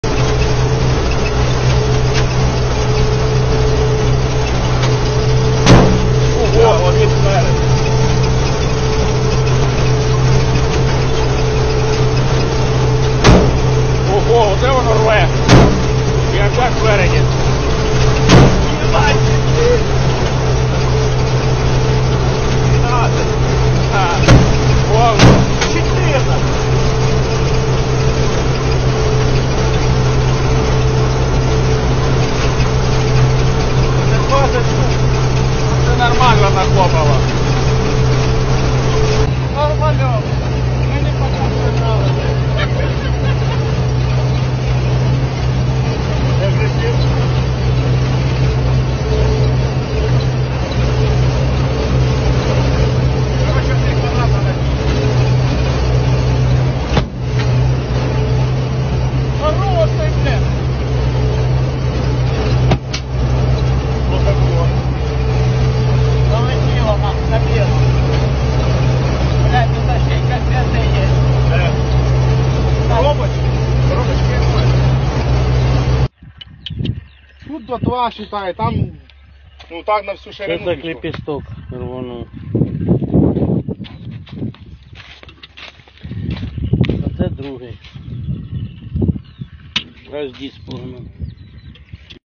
Реалії фермерів прифронтової України
Перед посівною, українські фермери жертвуючи життям та своєю технікою, розміновують поля!
Квітень 2024